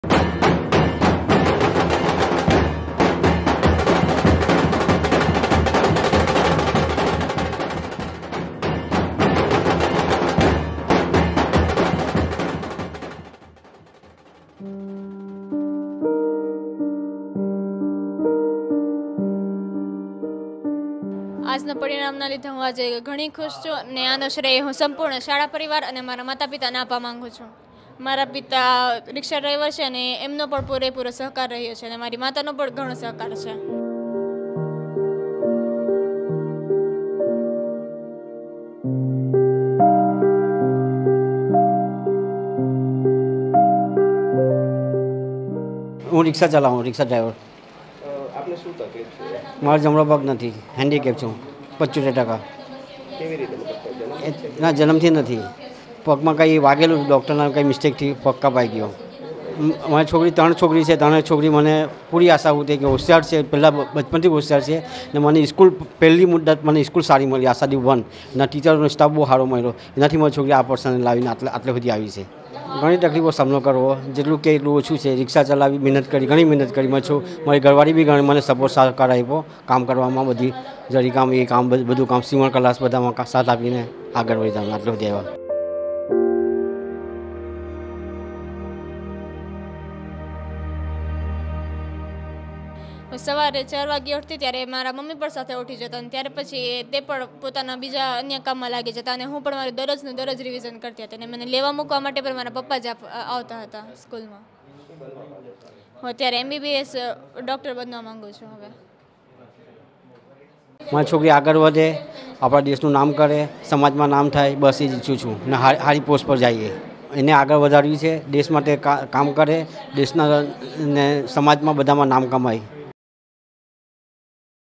Board Results: સુરતમાં વિકલાંગ રિક્ષાચાલકની દીકરીની સફળતા બાદ કેવી રીતે થઈ ઉજવણી
આ સુરત છે અને અહીં 10મા ધોરણના પરિણામ બાદ ઉજવણી થઈ રહી છે. તબસ્સુમ ખાન પઠાણને 94.50 ટકા આવ્યા છે. તેમની સફળતાના હીરો તેમના પિતા છે, જે વિકલાંગ છે છતાં રીક્ષા ચલાવે છે, દીકરીને ભણાવવા પિતા ઇલ્યાસ ખાને ઘણી મહેનત કરી છે. વિકલાંગ હોવા છતાં દીકરીનાં સપનાં સાકાર કરવા તેમણે ખૂબ મહેનત કરી છે.

તમે અમારી સાથે જોડાઈ શકો છો :

JioChat Channel : BBC Gujarati
ShareChat : bbcnewsgujarati